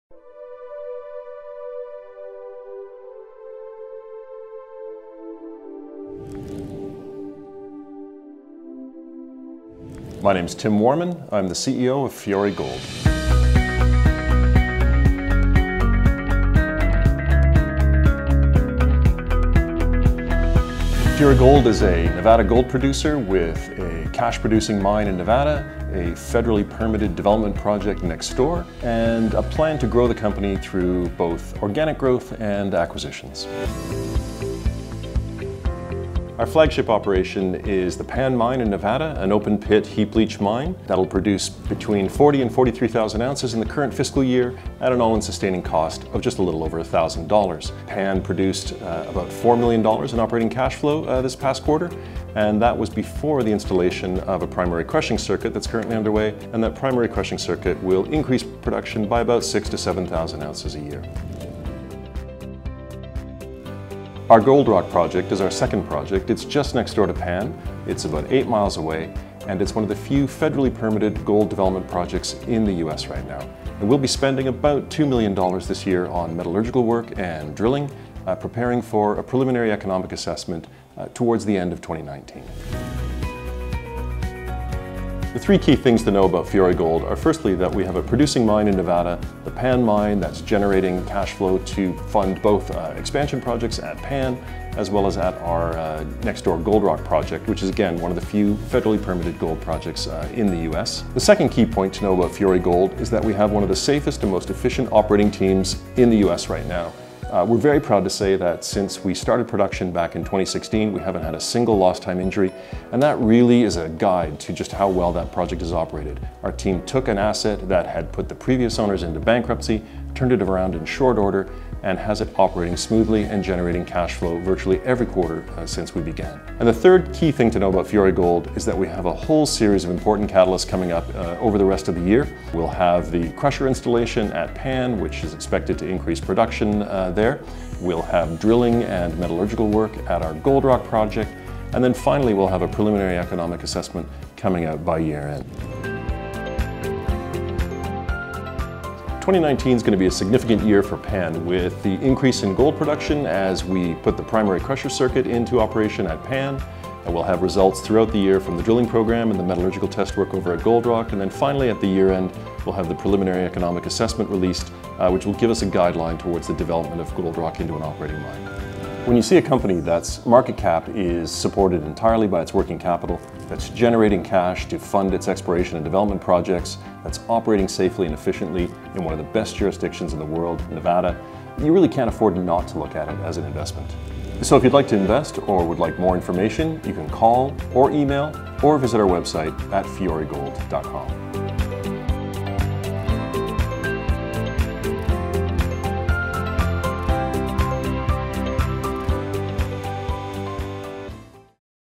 Fiore Gold Ltd.
Tim Warman, CEO & Director of Fiore Gold Ltd., discusses the company's projects and outlook for 2019.

Special thanks to PinnacleDigest.com for providing site footage, which has been adapted for this video.